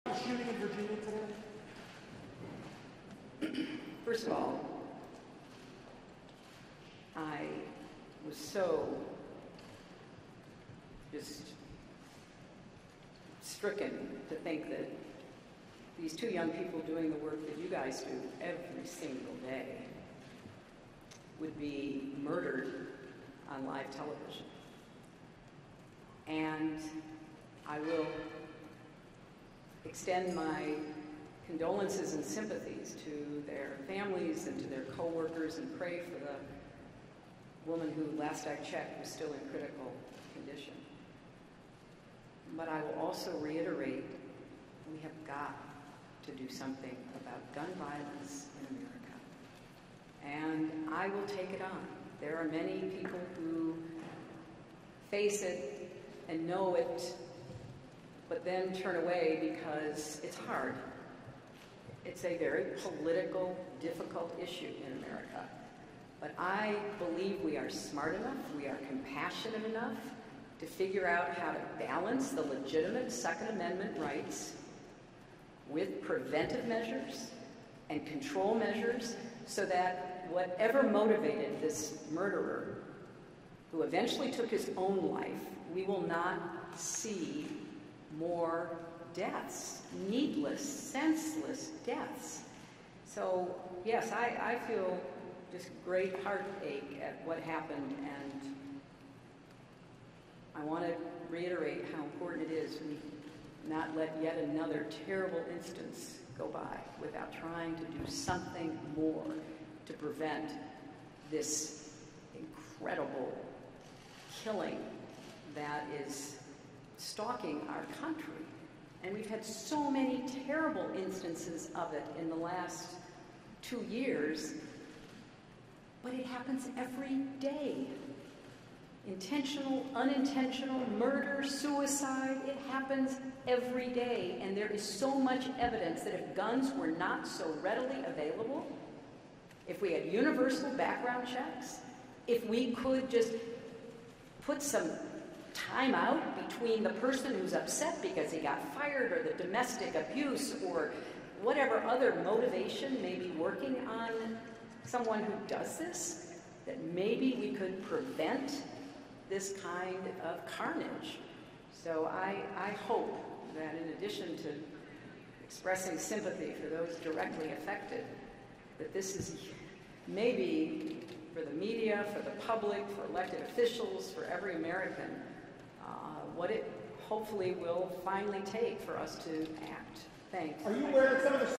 Hillary Clinton: “We have got to do something about gun violence in America.” (C-SPAN)
Hillary Clinton statement on on Virginia WDBJ shooting.